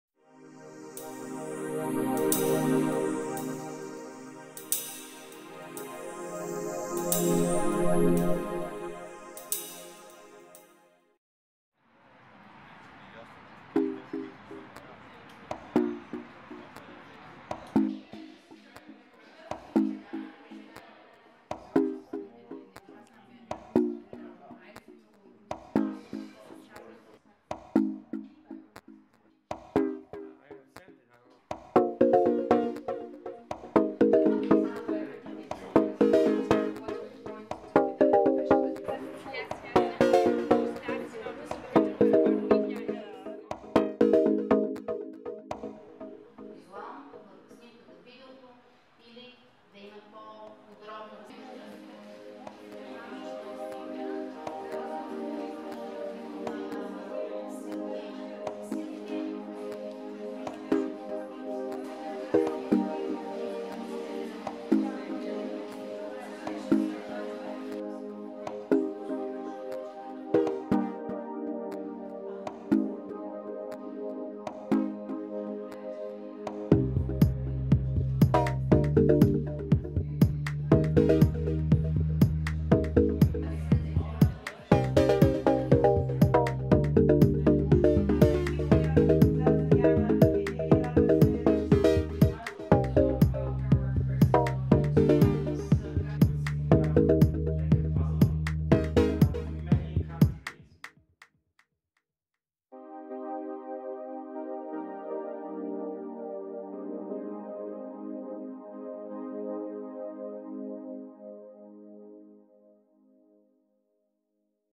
shifting walls - teacher training in Sofia
A warm welcome to the Erasmus+ shifting walls project!
shifting walls is about a critical engagement with the last 30 years of Europe's history through photo stories.